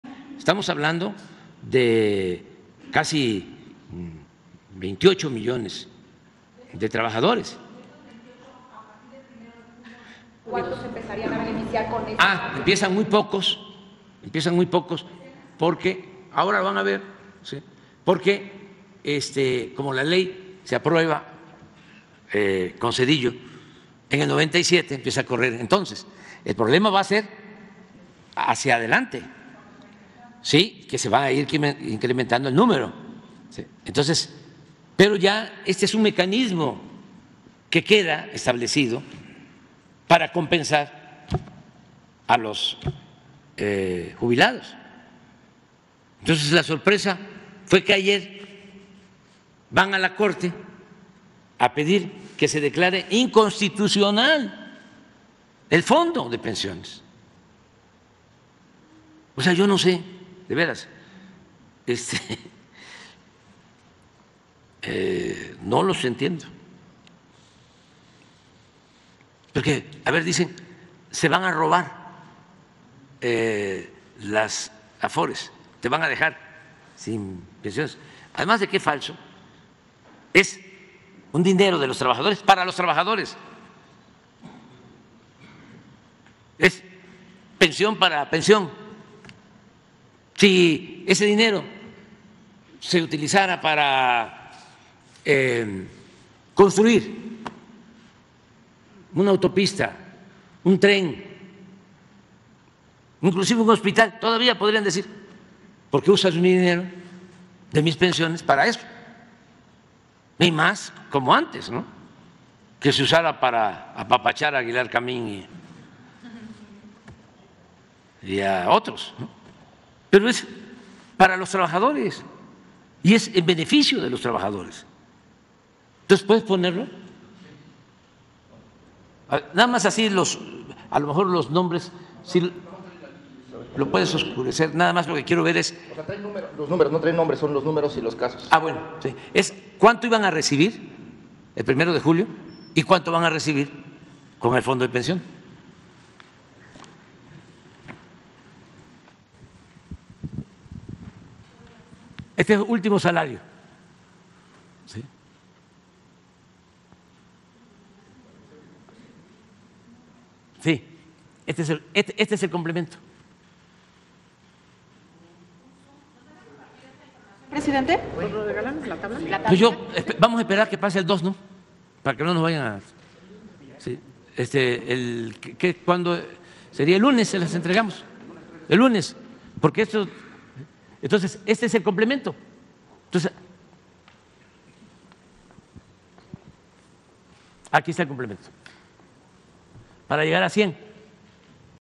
Legisladores de oposición interpusieron acción de inconstitucionalidad contra la ReformaDePensiones
Tras acción de inconstitucionalidad que legisladores de oposición interpusieron contra la ReformaDePensiones, AMLO muestra tabla del "complemento" que aportaría el Fondo del Bienestar para que jubilados cobren la totalidad de su último sueldo